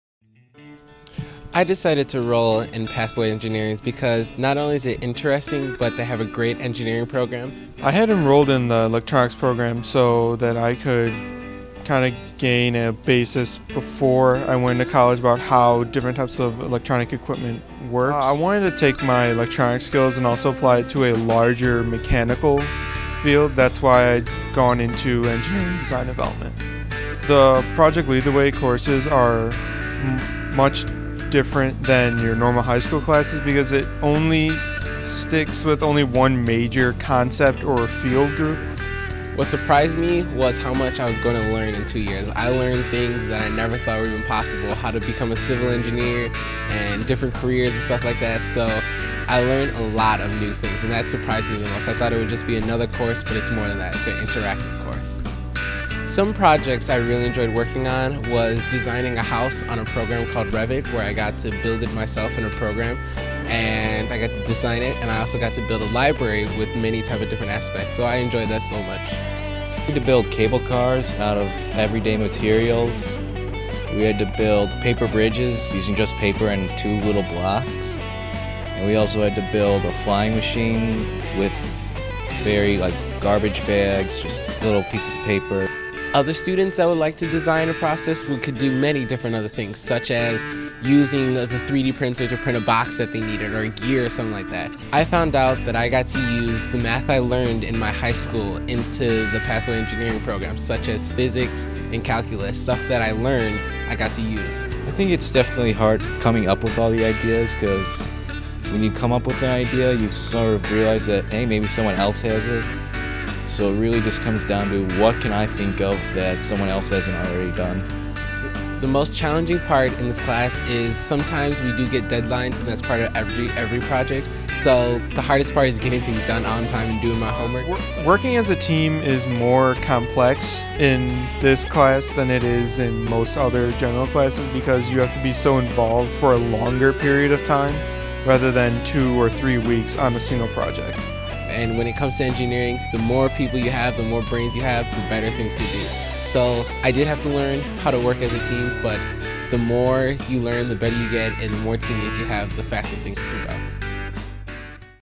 TCD-PLTW Pathway to Engineering
Students in the Pathway to Engineering series of courses at Technology Center of DuPage (TCD) talk about their experiences.
TCD is the elective high school campus for advanced career and technical education in DuPage County and Lyons Township (IL).